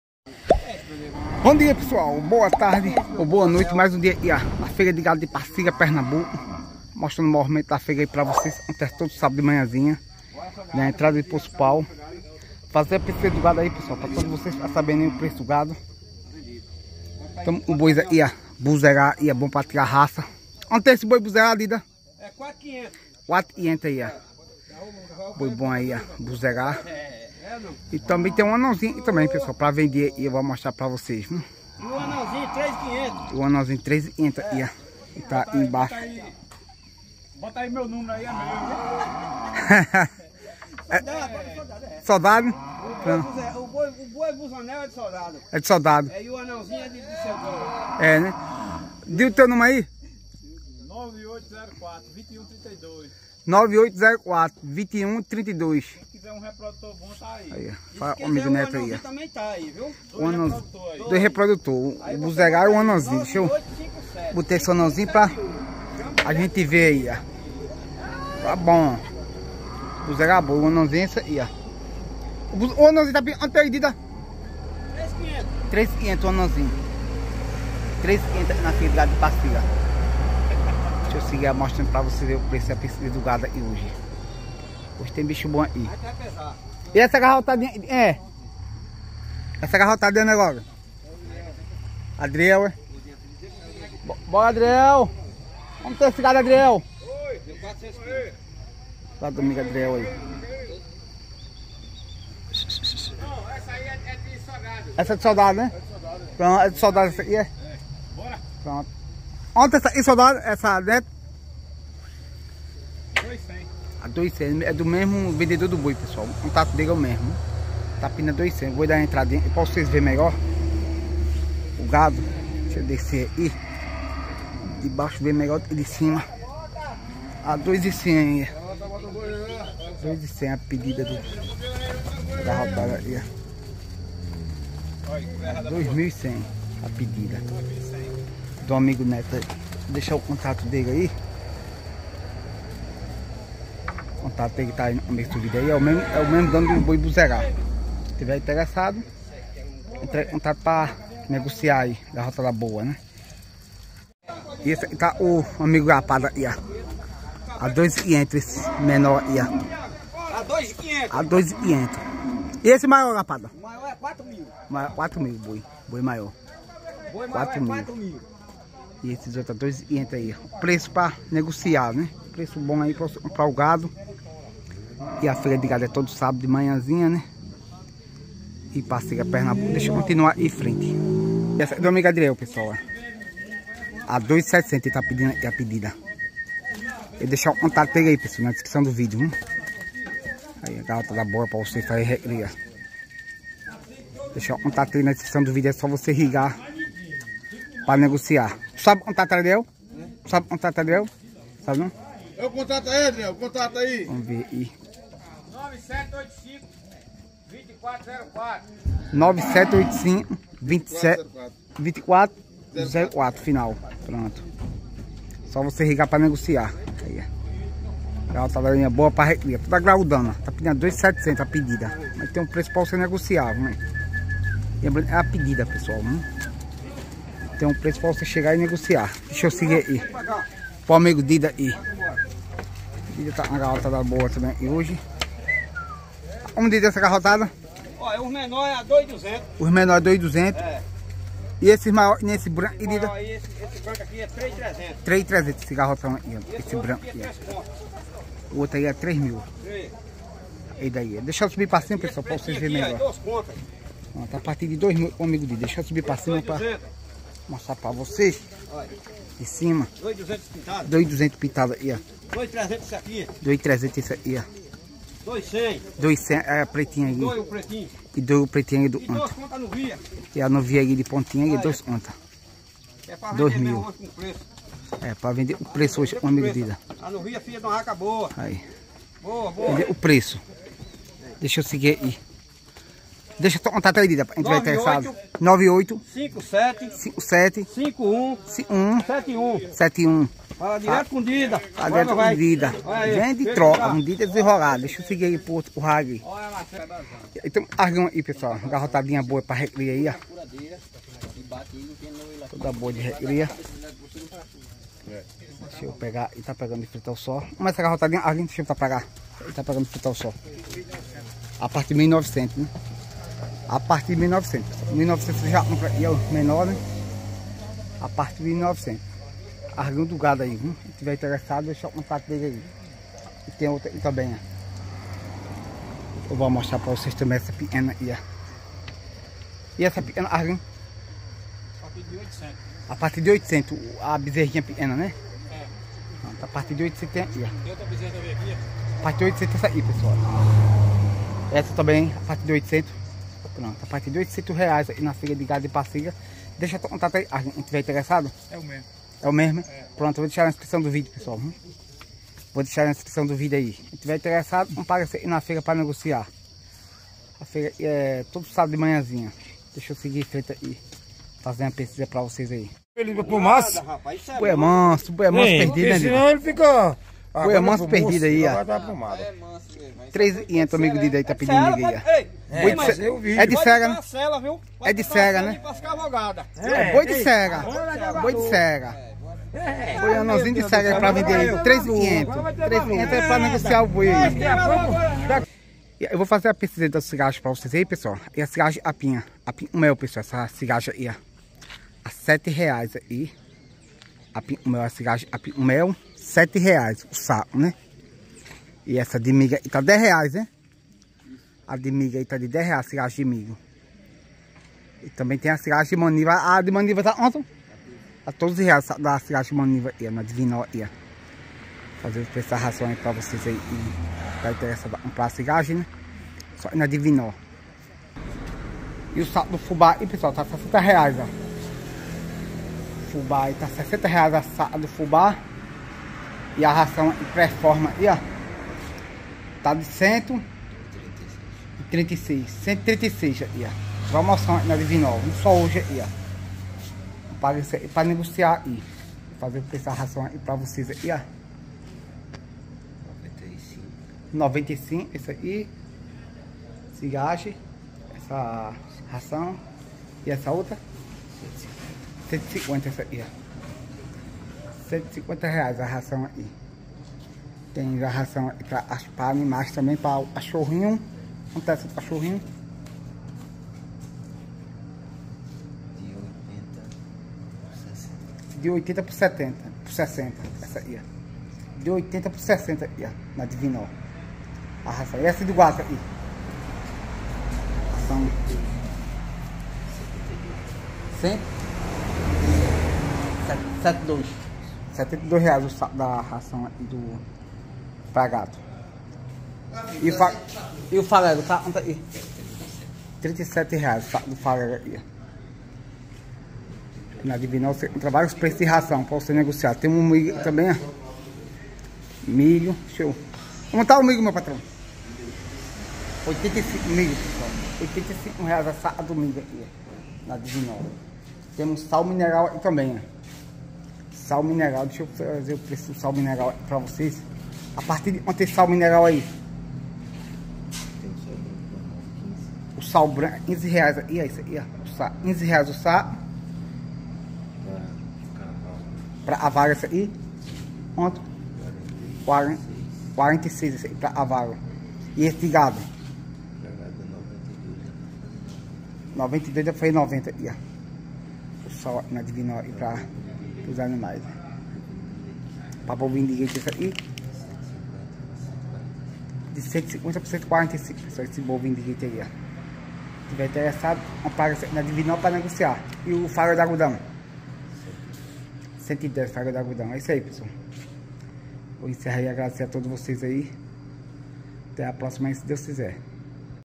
FEIRA DO GADO DE POÇO DO PAU PASSIRA PERNAMBUCO 12/08/2023
AJUDE O CANAL COM QUALQUER VALOR
JEFFERSON DOS SANTOS BARAUNA

@Criatório Fábio Santos

Ative às notificações

Feira do bode de Poço do Pau PE
Aos sábados